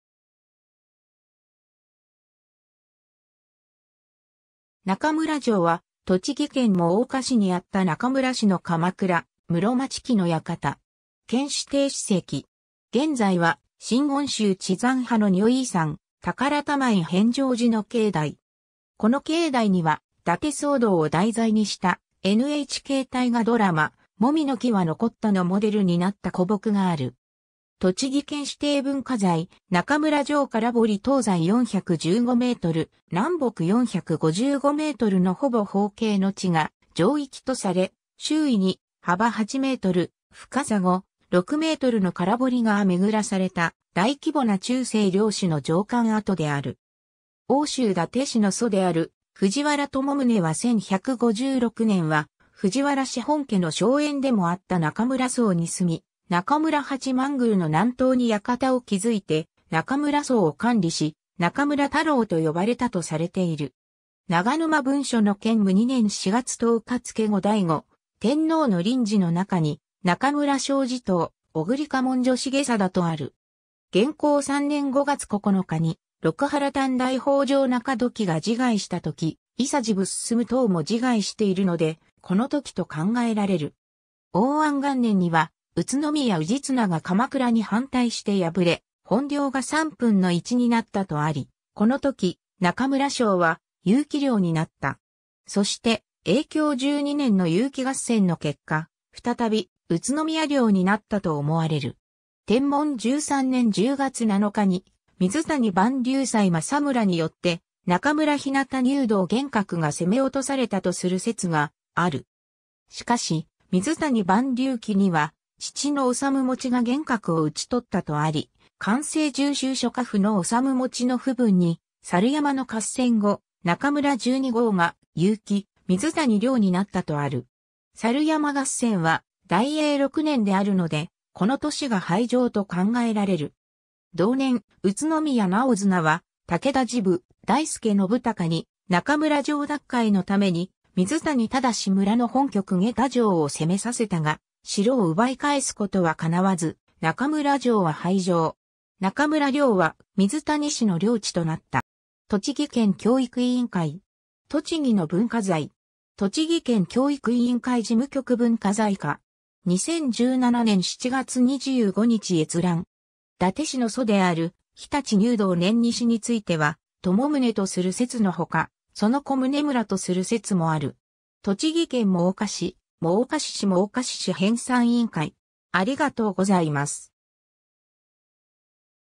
中村城 (下野国)
中村城（なかむらじょう）は、栃木県真岡市にあった中村氏の鎌倉、室町期の居館（または日本の城）。
県指定史跡。
現在は真言宗智山派の如意山寶珠院遍照寺の境内。
 この境内には、伊達騒動を題材にしたNHK大河ドラマ「樅ノ木は残った」（吉永小百合、平幹二朗等出演）のモデルになった古木がある。
 栃木県指定文化財・中村城空堀 東西415メートル、南北455メートルのほぼ方形の地が城域とされ、周囲に幅8メートル、深さ5,6メートルの空堀がめぐらされた大規模な中世領主の城館跡である。
奥州伊達氏の祖である藤原朝宗は1156年（保元元年）は藤原氏本家の荘園でもあった中村荘に住み、中村八幡宮の南東に館を築いて中村荘を管理し、中村太郎と呼ばれたとされている。
 「長沼文書」の建武2年（1335年）4月10日付け後醍醐天皇の綸旨の中に「中村庄地頭小栗掃部助重貞」とある。
元弘3年（1333年）5月9日に六波羅探題北条仲時が自害したとき、伊佐治部丞等も自害している（『太平記』）ので、このときと考えられる。
 応安元年（1368年）には宇都宮氏綱が鎌倉に反対して敗れ、本領が3分の1になった（『鎌倉大日記』）とあり、このとき中村庄は結城領になった。
そして、永享12年（1440年）の結城合戦の結果、再び宇都宮領になったと思われる。
 天文13年（1544年）10月7日に水谷蟠龍斎正村によって中村日向入道玄角が攻落された（『中村沿革誌』）とする説がある。
しかし、「水谷蟠龍記」には父の治持が玄角を討取ったとあり、「寛政重修諸家譜」の治持の譜文に、猿山の合戦後、中村十二郷が結城・水谷領になったとある。
猿山合戦は大永6年（1526年）であるので、この年が廃城と考えられる。
 同年、宇都宮尚綱は武田治部大輔信隆に中村城奪回のために水谷正村の本拠久下田城を攻めさせたが、城を奪い返すことはかなわず、中村城は廃城。
中村領は水谷氏の領地となった。
 （城主名は中村沿革に基づく表記） ^ 栃木県教育委員会. “とちぎの文化財”. 栃木県教育委員会事務局文化財課. 2017年7月25日閲覧。
 ^ 伊達氏の祖である常陸入道念西については、朝宗とする説のほか、その子宗村とする説もある。
 ^ 栃木県真岡市「真岡市史」真岡市史編さん委員会